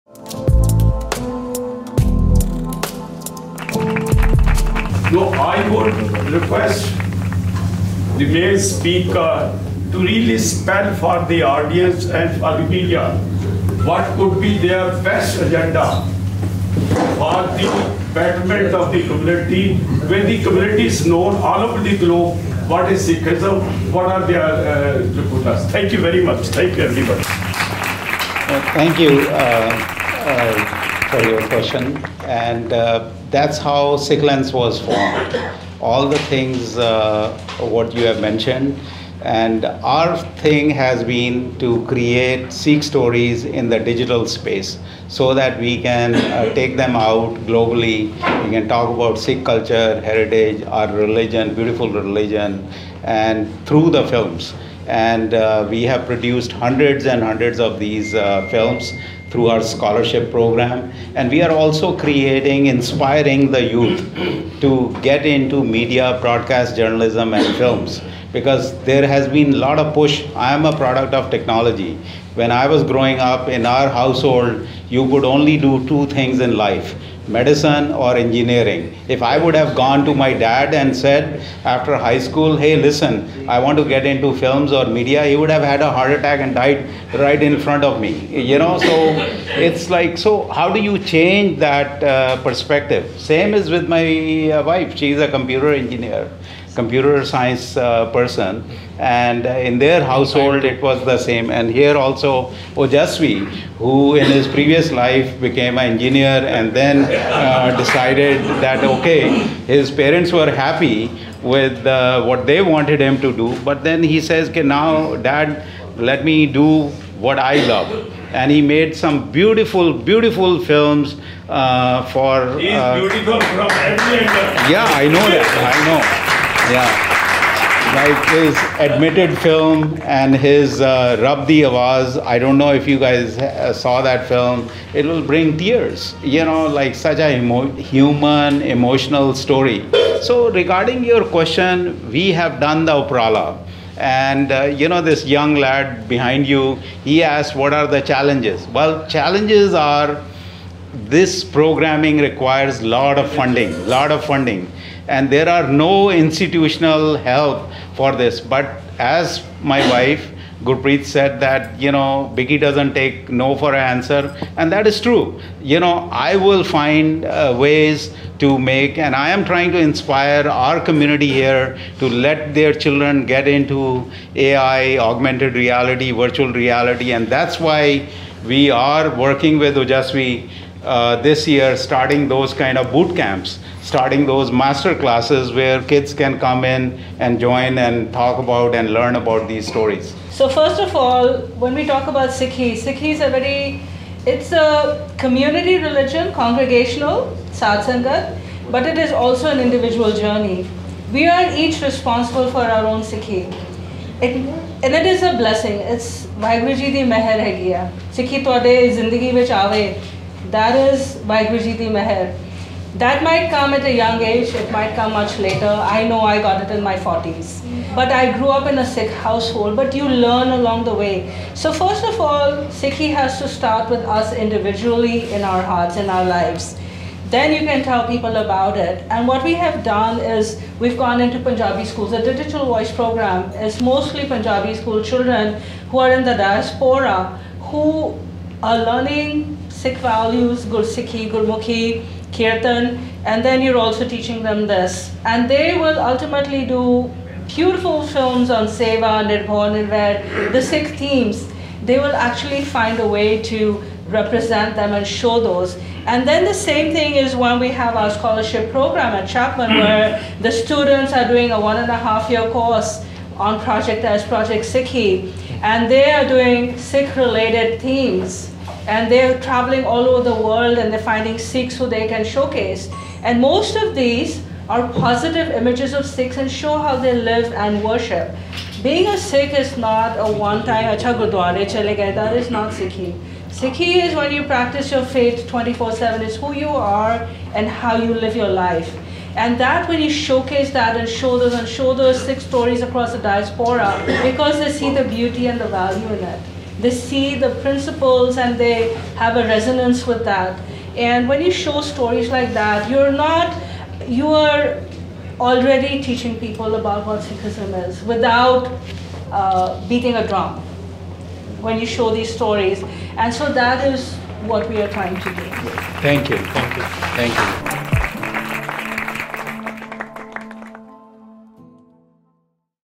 Changing the perspective of Society towards filmmaking | Sikhlens India 2024 Festival in Chandigarh
Highlights from the Press Conference of Sikhlens: Sikh Arts & Film Festival 2024 (India Chapter).

Location: Chandigarh Press Club

Festival Founder: Bicky Singh
Festival Director - India: Shri Ojasweee Sharma

Sikhlens Produced in India by Pinaka Mediaworks & Rolling Frames Entertainment (RFE TV)

The Cybersikh Organization - Sikhlens. CA, United States of America.
Pinaka Mediaworks LLP and Rolling Frames Entertainment